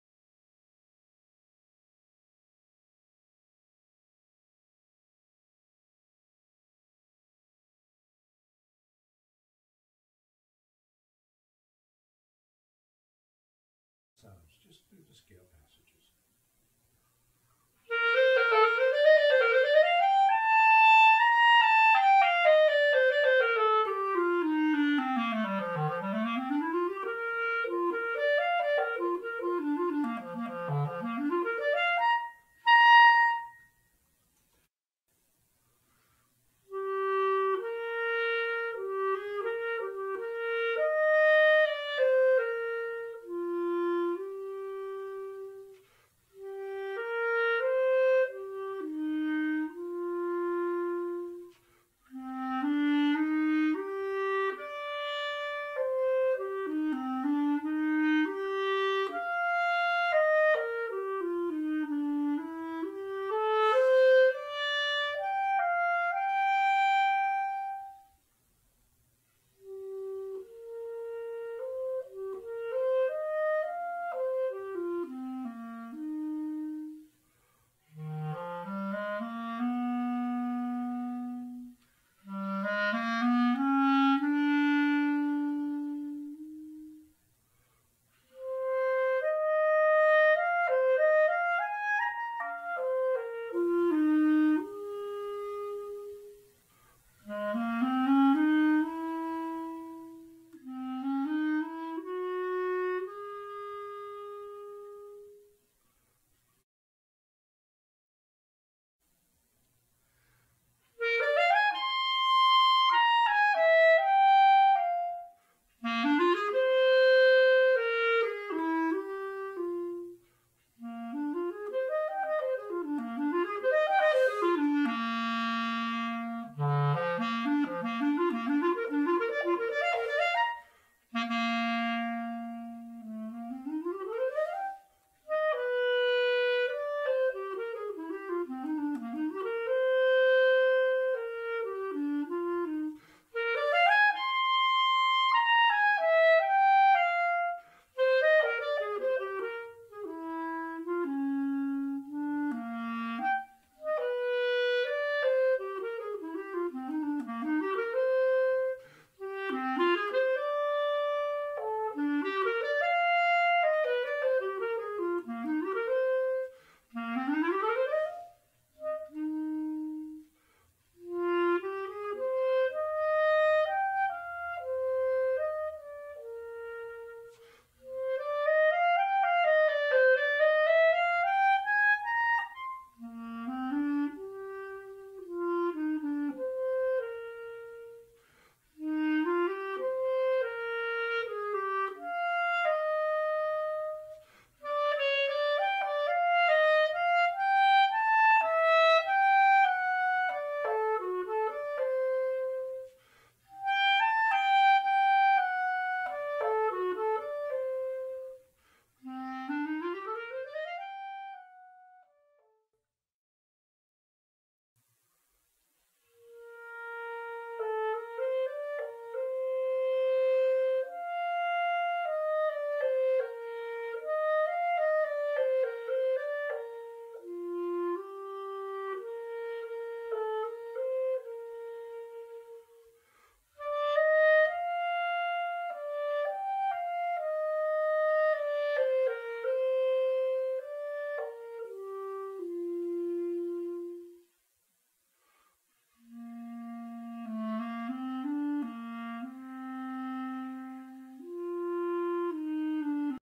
Boosey & Hawkes 8-10 Clarinet Audio Sample
THE VINTAGE CLARINET:  Here is a short audio sample displaying the charms of fully restored and voiced Boosey and Hawkes clarinet.